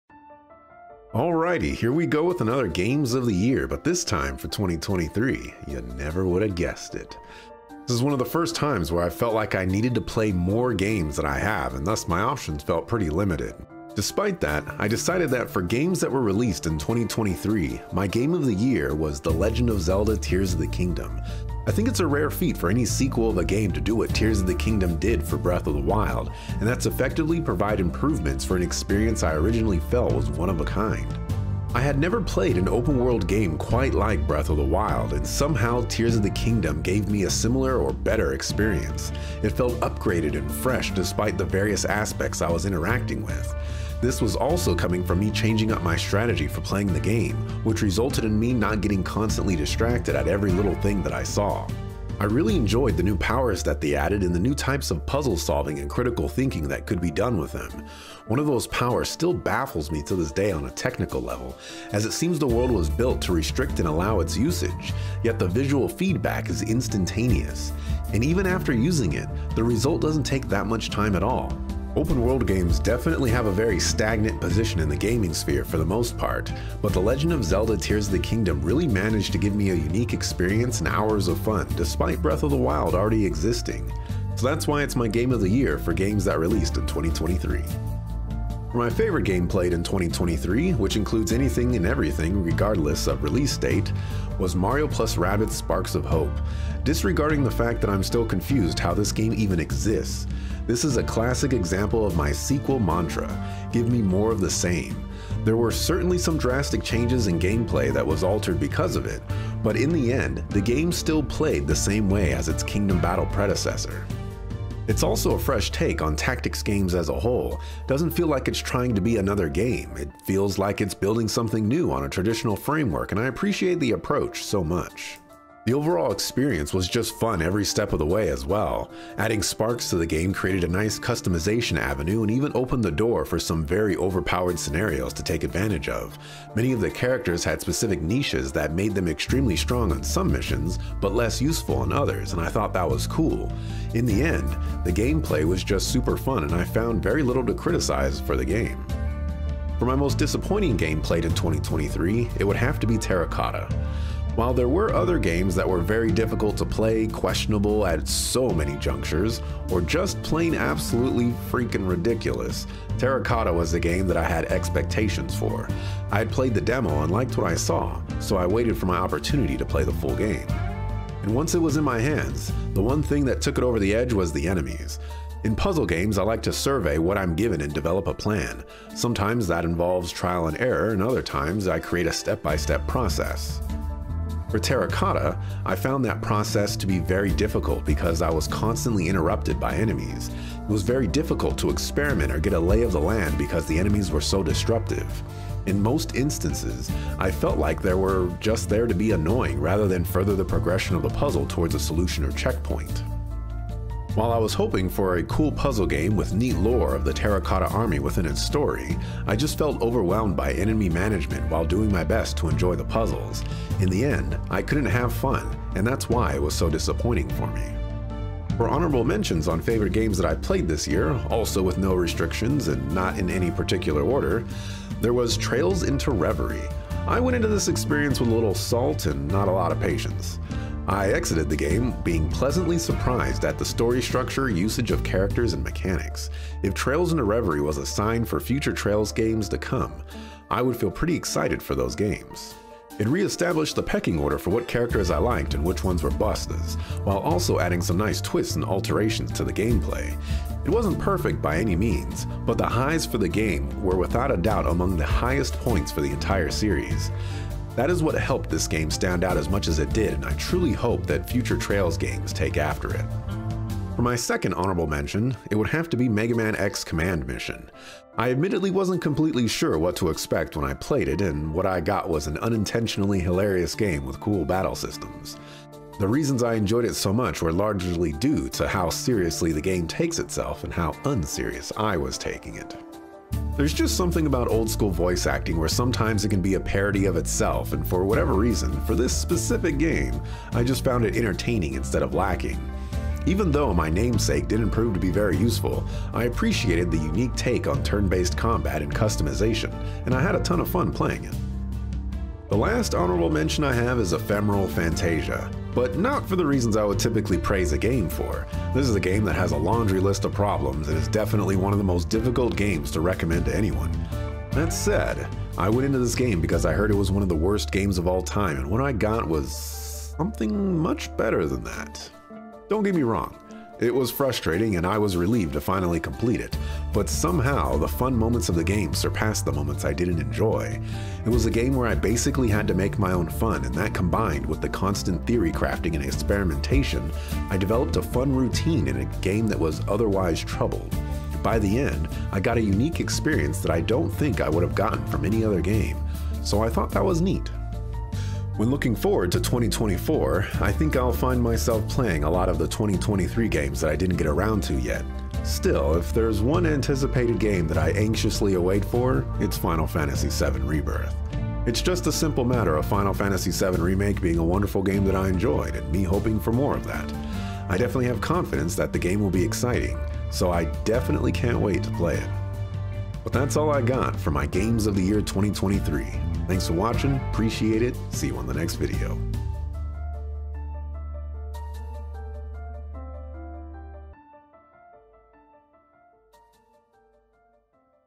2023 - Axelrod's Games of the Year
Another great year in gaming! I played a decent chunk of games throughout the year and in this video, I will highlight five categories of games I played!

A game released this year that I played and enjoyed above all others.

*** Favorite Game Played ***
Any game played during this year that I enjoyed, excluding game of the year.

*** Most Disappointing ***
A game that failed to meet my expectations or in some cases, the game I felt was very poor in most crucial categories of game development.

*** Honorable Mentions ***
These are games that are ranked just below game of the year and narrowly missed the spot for one reason or another.

*** Most Anticipated ***
Games releasing next year or beyond that I'm looking forward to playing.

For the lazy, my choices were the following:

Game of the Year: The Legend of Zelda: Tears of the Kingdom
Favorite game: Mario + Rabbids: Sparks of Hope
Disappointing Game: Terracotta
Honorable Mentions: Trails Into Reverie, Megaman X: Command Mission, Ephemeral Fantasia

Most Anticipated: Final Fantasy 7 Rebirth